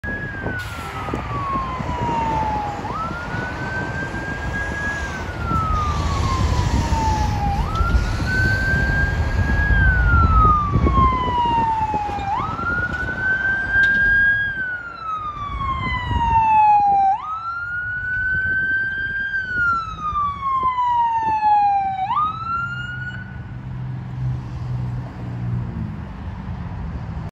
Med Ex Ambulance Transporting Code 3.
Transporting to Gottlieb Hospital.